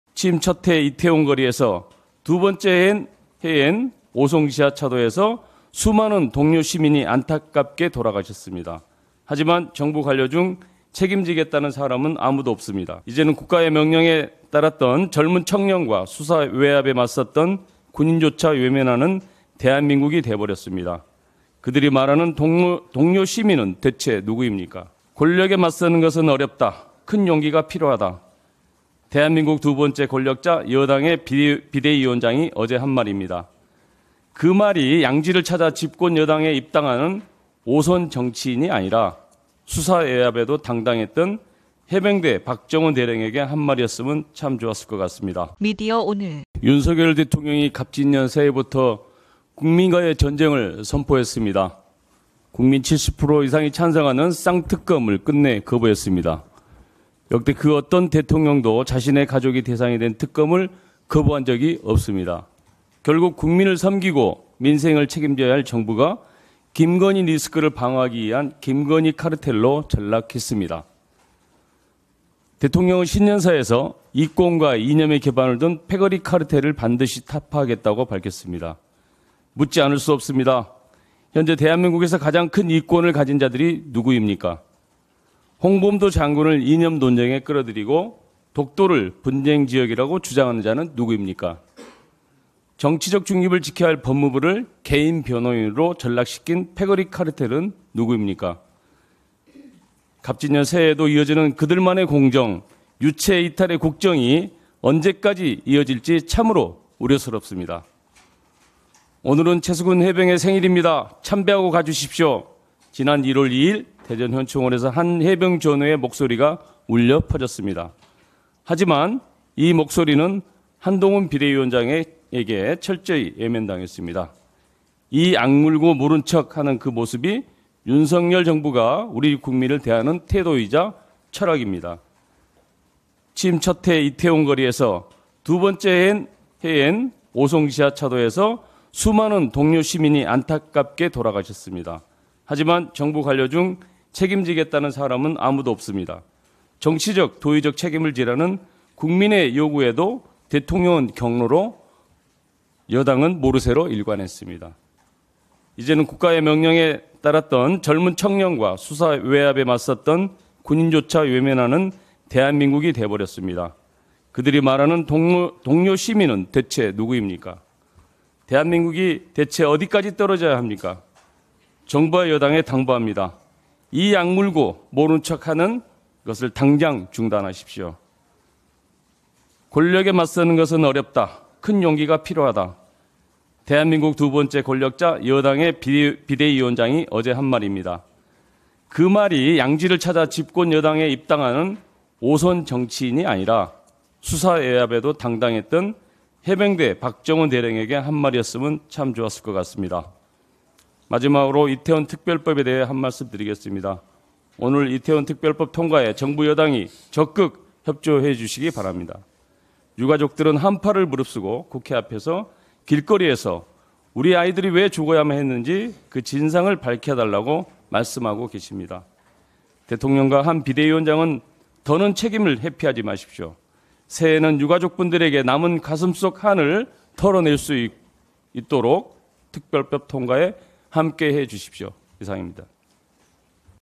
문진석 “권력에 맞서는 것 용기 필요? 양지 찾은 5선 아닌 박 대령에 해야”
[영상] 문진석 “권력에 맞서는 것 용기 필요? 양지 찾은 5선 아닌 박 대령에 해야”
“수사 외압 맞선 군인조차 외면하는데 동료 시민은 대체 누구인가?”
“이태원과 오송에서 동료 시민들 돌아가셨지만 정부 관료 중 책임지겠다는 사람 없어”
이권과 이념 카르텔? 가장 큰 이권 가진 자와 홍범도 장군 이념 논쟁 누가 하나?
9일 민주당 원내대책회의 모두 발언